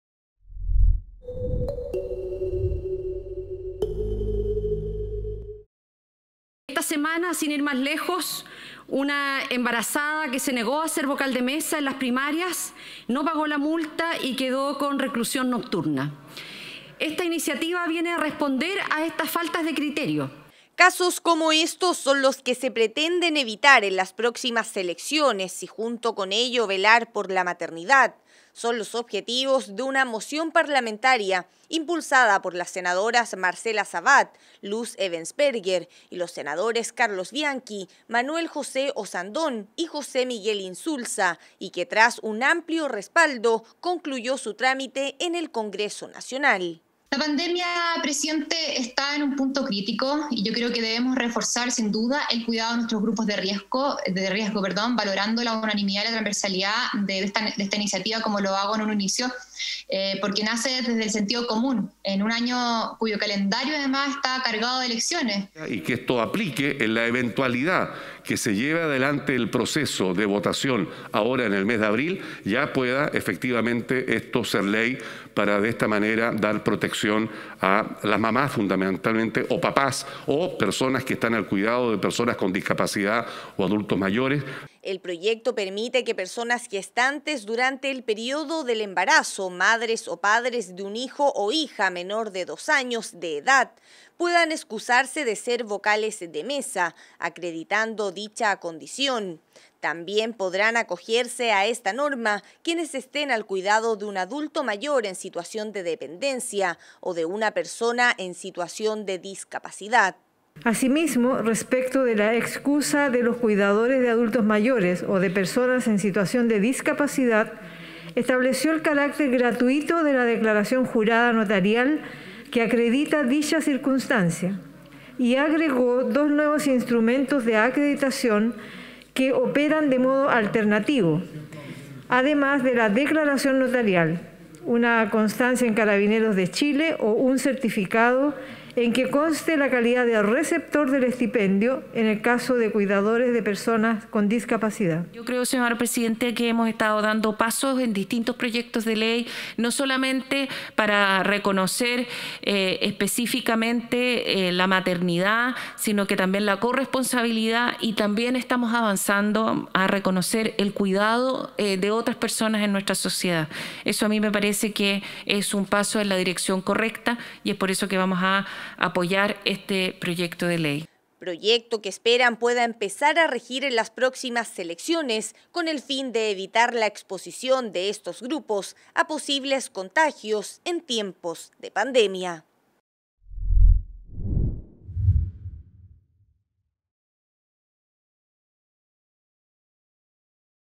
Senado Noticias - Proyecto permitirá excusarse de ser vocal de mesa.
Podrán acogerse madres, padres, embarazadas y cuidadoras.